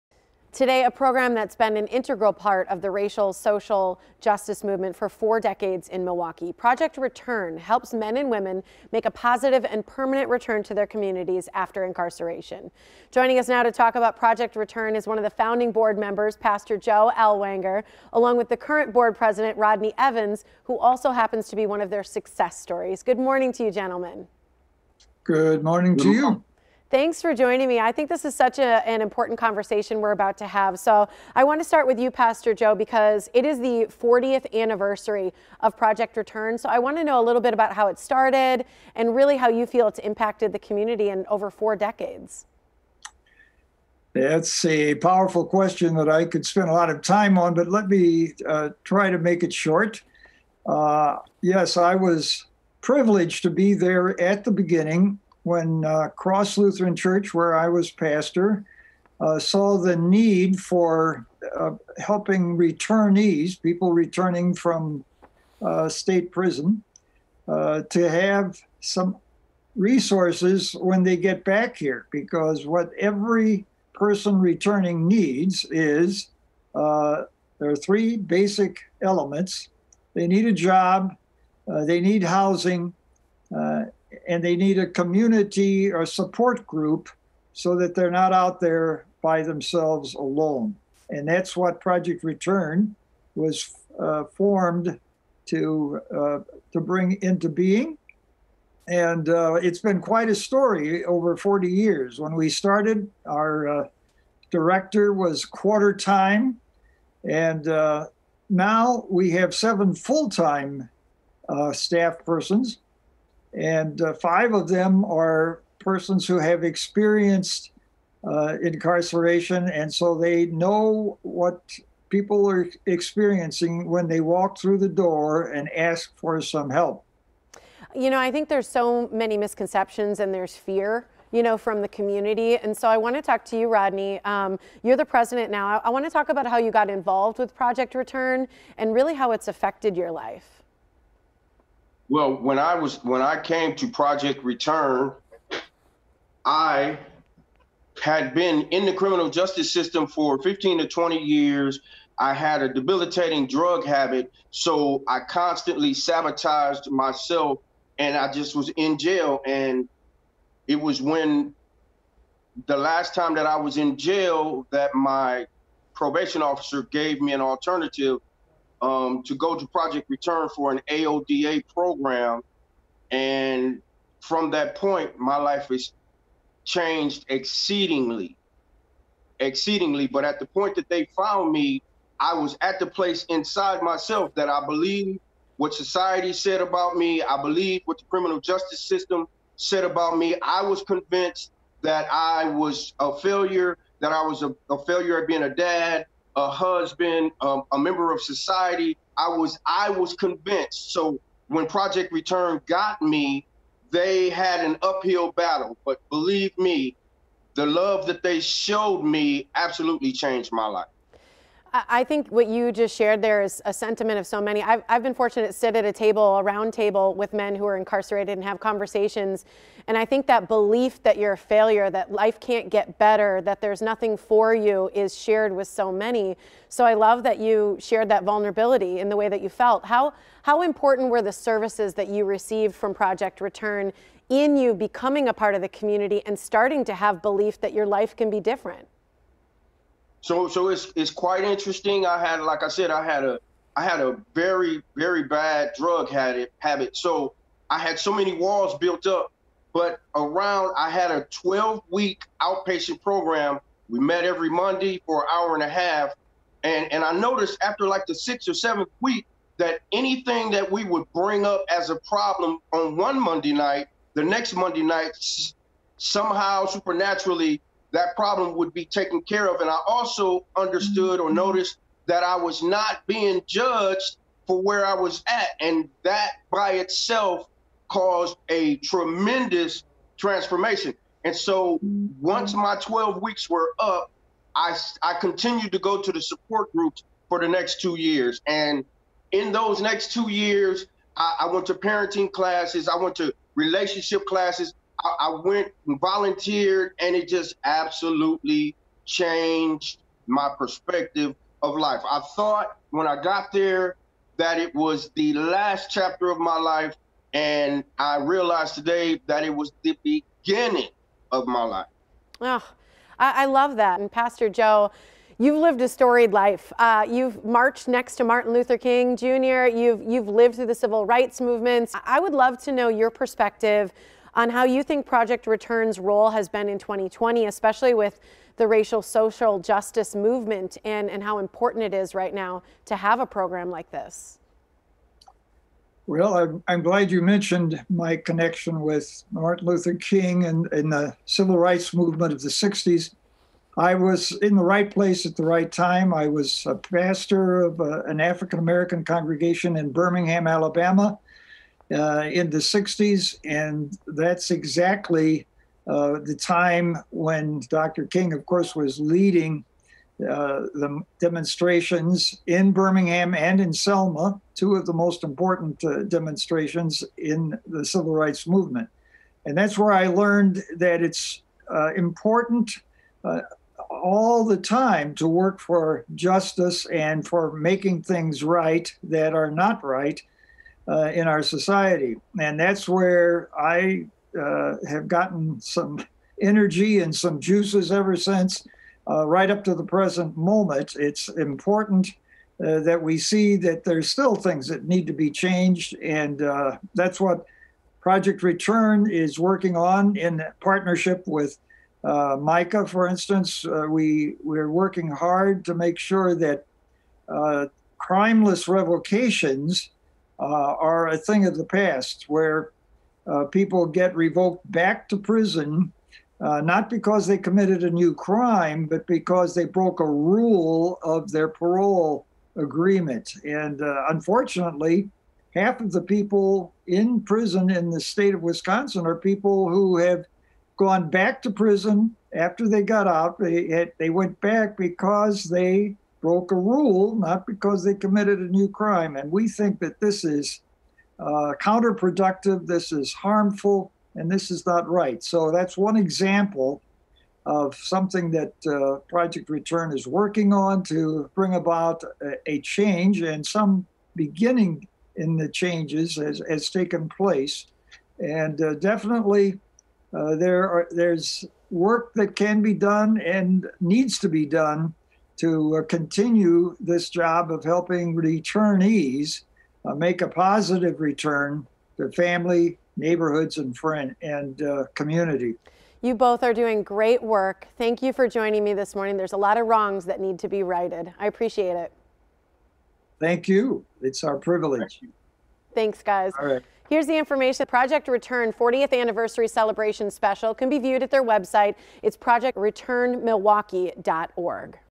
40 Years of Service to the Formerly Incarcerated
Project RETURN has been an integral part of the racial social justice movement for four decades, helping men and women make a positive, permanent return to their communities after incarceration. To address the issue of high recidivism in Milwaukee, Project RETURN walks alongside ex-incarcerated people by providing assistance with employment, housing, drug and alcohol counseling, life skills training and more. Joining us to discuss their mission are Founder and Board Member, Pastor Joe Ellwanger, and Board President, Rodney Evans.  For more information on Project RETURN and to view their 40th Anniversary Celebration Special, visit ProjectRETURNMilwaukee.org.